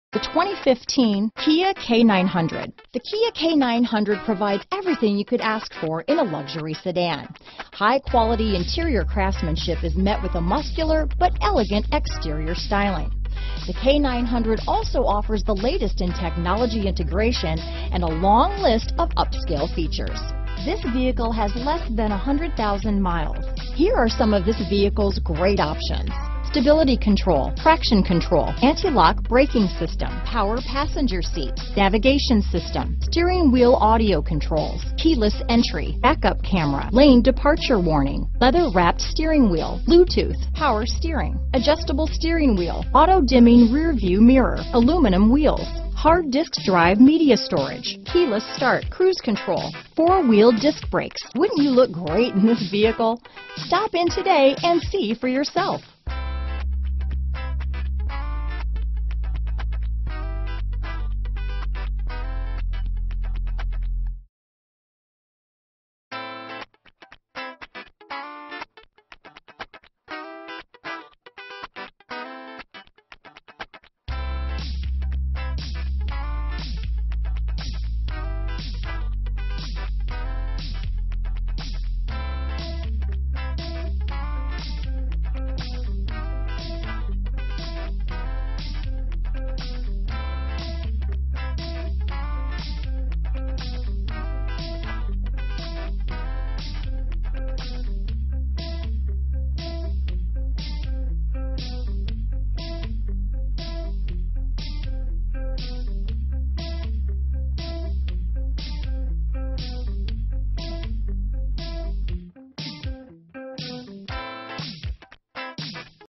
2015 Kia K900 Davenport, Celebration, Kissimmee, Lakeland, Sebring, FL PF6024625
Mineral Silver Used 2015 Kia K900 available in Davenport, Florida at Miracle Toyota. Servicing the Celebration, Kissimmee, Lakeland, Sebring, FL area.

Miracle Toyota
37048 US-27

Recent Arrival! AutoCheck Certified / Accident Free, 1 Owner.2015 Kia K900 Premium 5.0L V8 DGI DOHC Mineral Silve.r